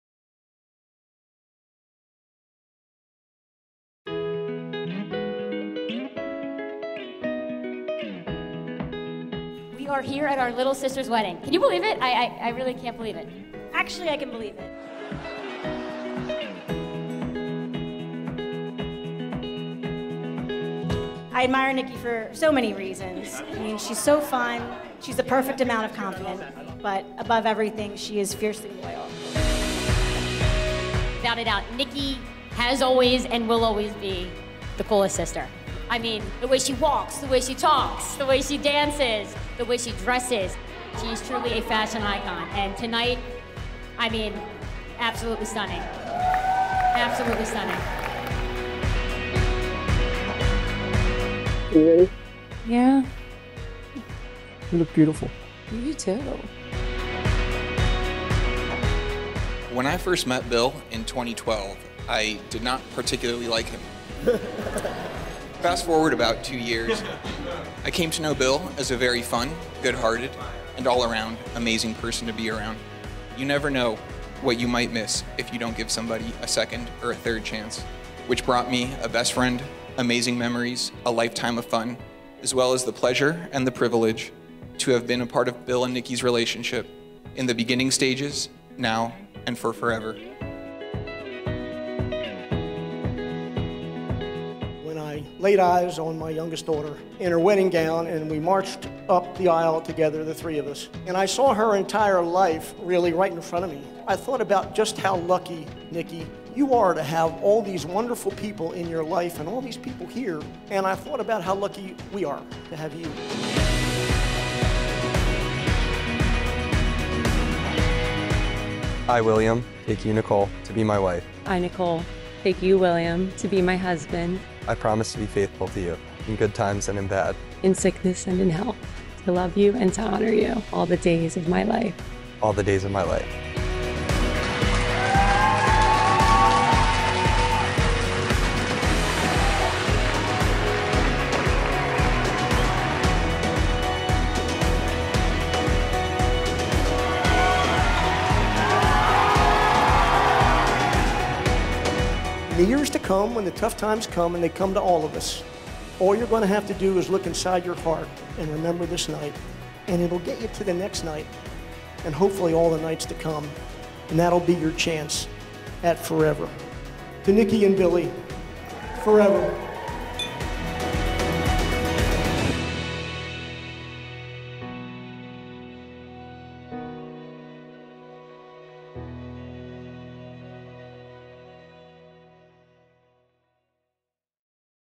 Nikki and Bill's CinemaCake Wedding Coming Soon Trailer
Coming Soon: CinemaCake Filmmakers
Photography: Anerino Originals
Coordinator: The Styled Bride
Preparations and Reception Venue: Union League
Ceremony Venue: Cathedral Basilica of Saints Peter and Paul
Florist: Petals Lane
Hair Stylist: Amanda D'Andrea Hair
Makeup Artist: Beke Beau Make-up Studio
Entertainment: BVTLive Jellyroll